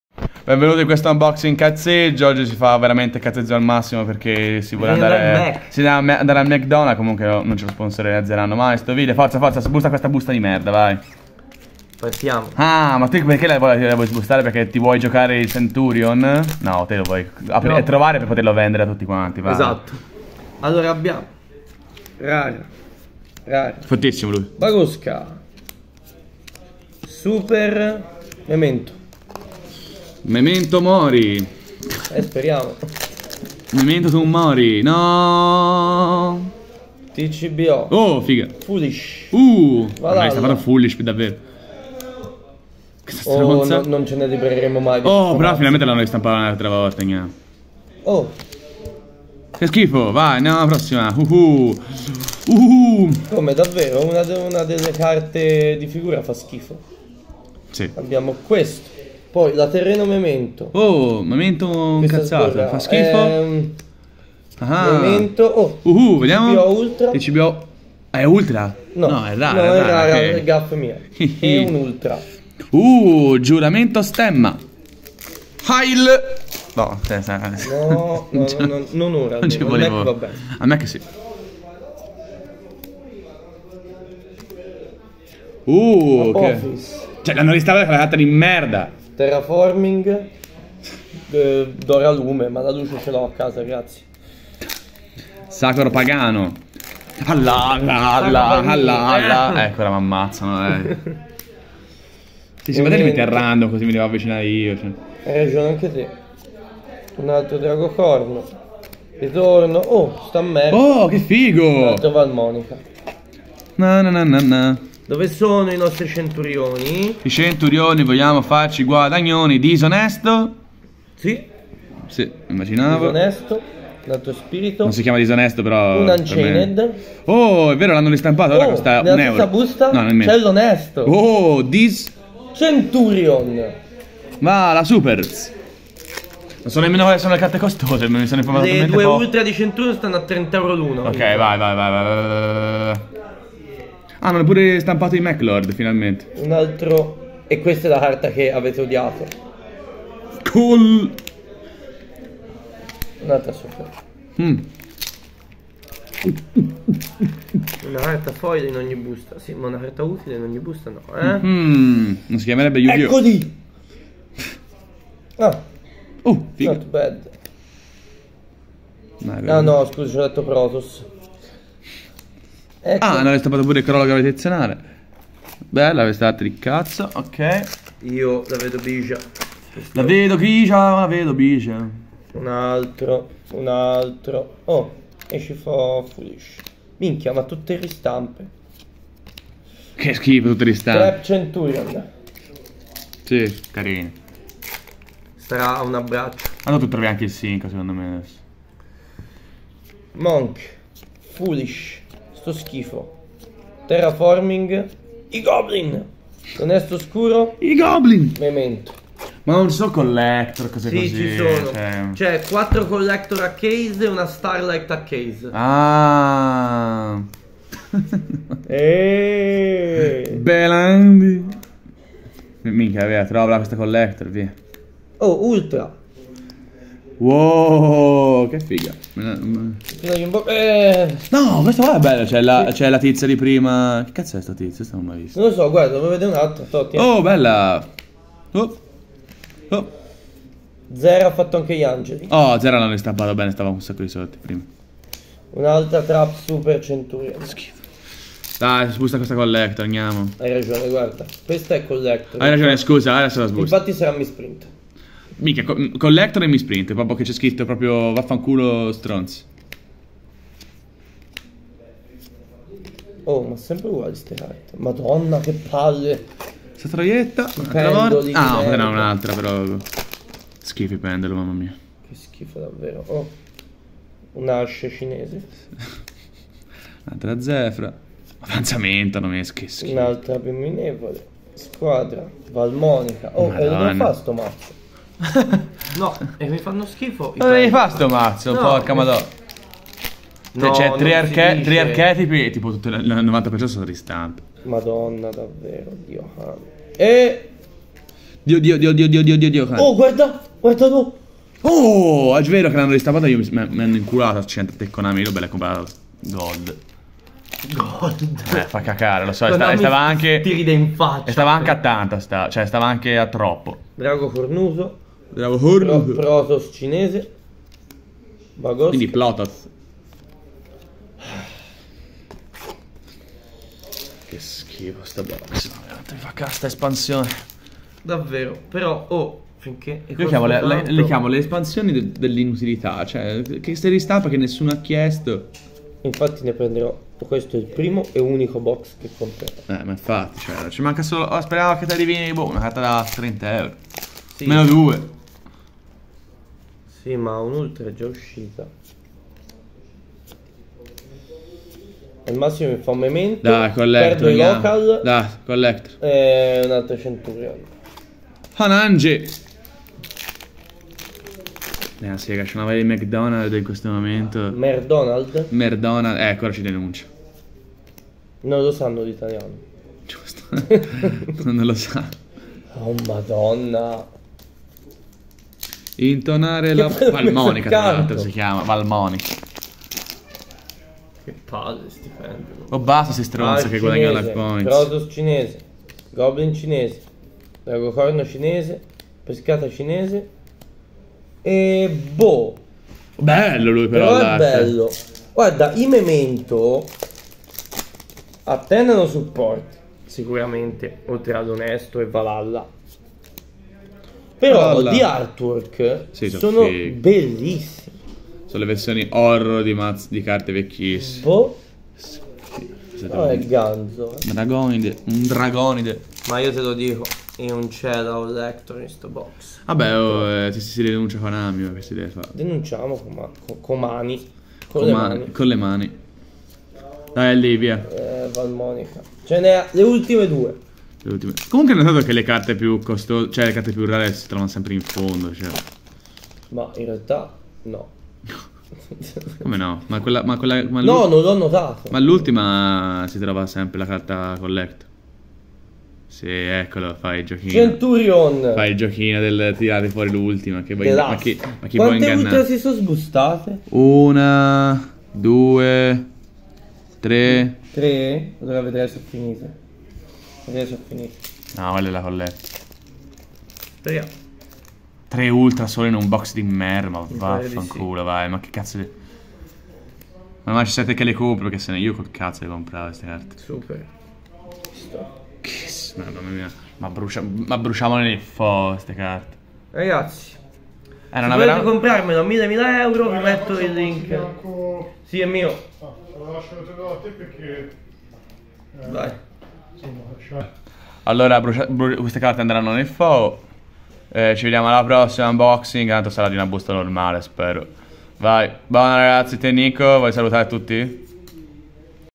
OPENING PACK (Poco serio) VASM - Che busta di merda bleah!
Deuna sega che merda sta busta.
Unboxing cazzeggio ma stavolta con 1 BOX di merda dove hanno fatto ristampe di carte che tutti avevano e un archetipo che costa una fracca e ancora non si sa come sarà, un saluto a stefano che ci ha speso 300+ euro.
Ogni Sabato alle 15:00 in Via Masini al GG -GoodGame Local, venite a trovarci per conoscerci.

We thank GG GoodGame Empoli(FI) Italy for the Support